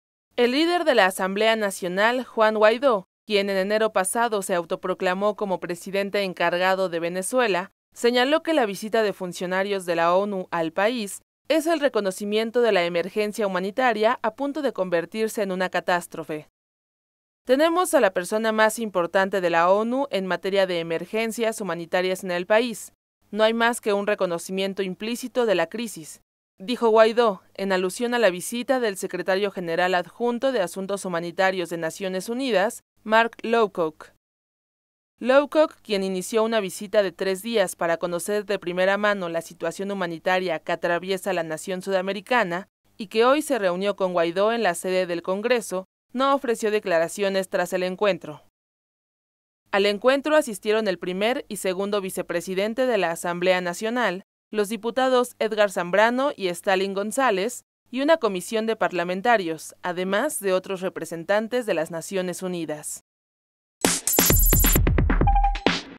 Reconoce ONU emergencia humanitaria: Guaidó
El líder de la Asamblea Nacional, Juan Guaidó, quien en enero pasado se autoproclamó como presidente encargado de Venezuela, señaló que la visita de funcionarios de la ONU al país es el reconocimiento de la “emergencia humanitaria a punto de convertirse en una catástrofe”.